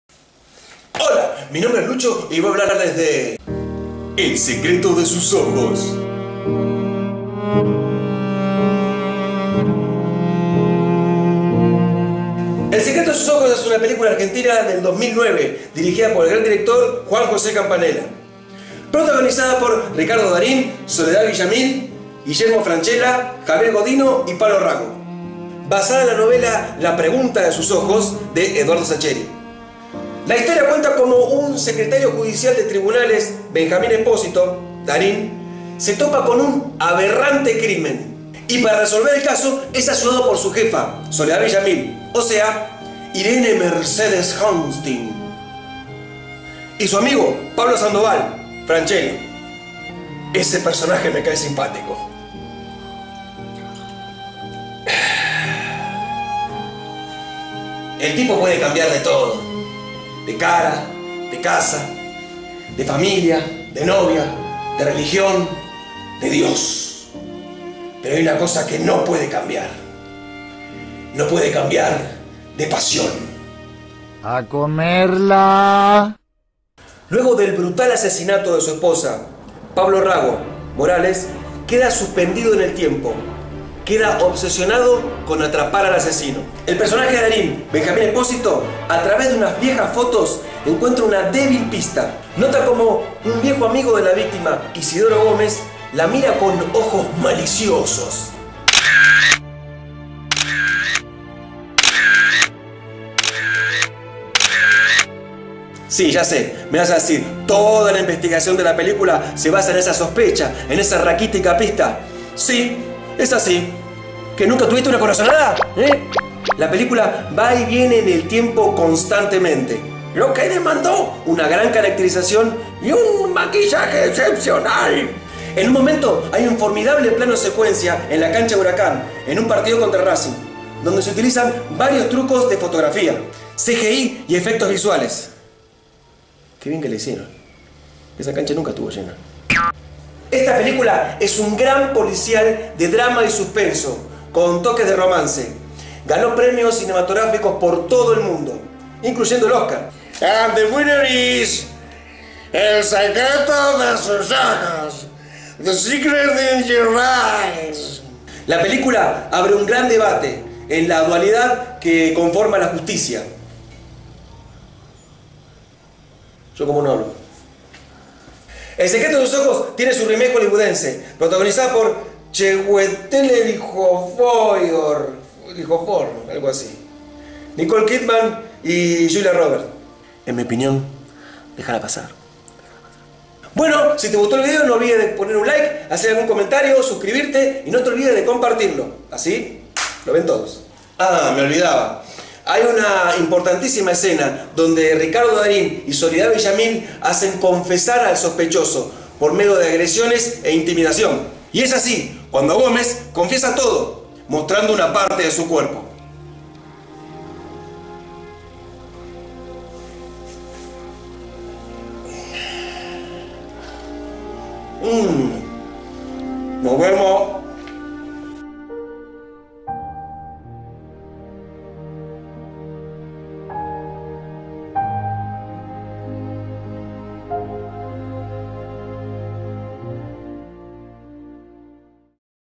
EL SECRETO DE SUS OJOS - SEGÚN LUCHO
VLOG DE COMEDIA BASADO EN LA PELICULA EL SECRETO DE SUS OJOS.
...LAS OPINIONES DICHAS EN EL VIDEO SON PURO ENTRETENIMIENTO